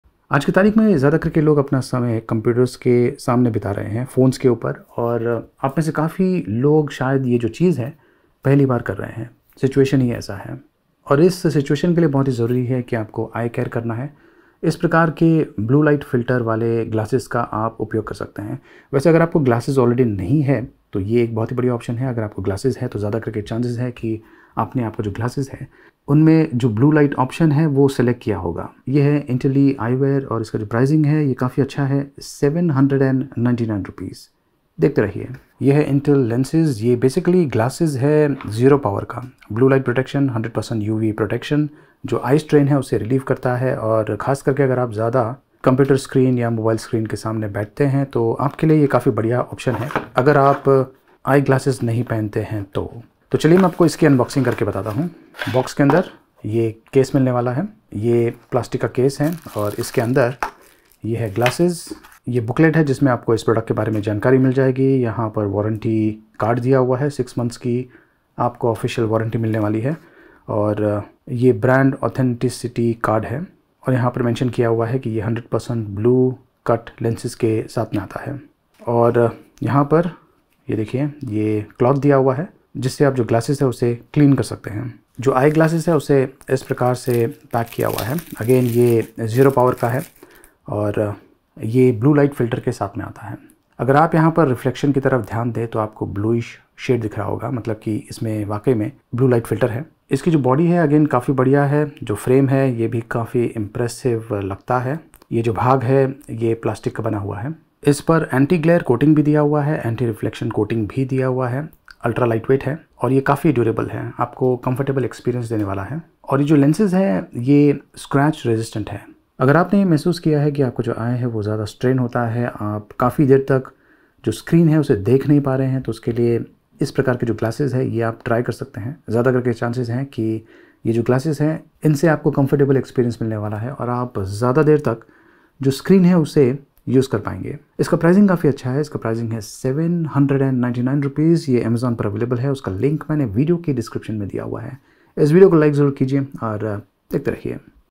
You need this Eye Glasses with Blue light filter lens - Intellilens Eye Protection
These tough times you need more protection for your eyes, the Intellilens Eye Protection glasses is the best you can get for around Rs. 800.

specs

Care Instructions: Wash under running water and wipe dry with cleaning cloth
Fit Type: Overall
Material and Frame Size: Our square anti glare bllue cut glasses are made from high quality plastic material, the full frame spectacles are free size and designed to fit with utmost comfort. the standard size dimension for the product is 52 mm-16 mm-138 mm.
Eye care: square blue light filter lens is upgraded from normal computer glasses, perfect for you who uses computer and phones all day for work and was complaining of eye fatigue and pain, redness. you will notice a difference.these are worth it to save your eyes from fatigue with electronic screen use. help with eye strain from computer, tablet, smartphone use for extended periods of time.
High quality design: lightweight plastic frame, blue light filter lenses with exquisite temples. all the details make our square full rim eyeglasses more durable enough for long time use & ensure you a perfect performance.
Transparent lens: multi layer blue filter coating is much more effective than non-coated glasses. this filters out almost all of blue light while letting you see clearly and precisely. great for close work when you want to see details. filters out annoying glare for better safety when driving. great for walks and enjoying the outdoors.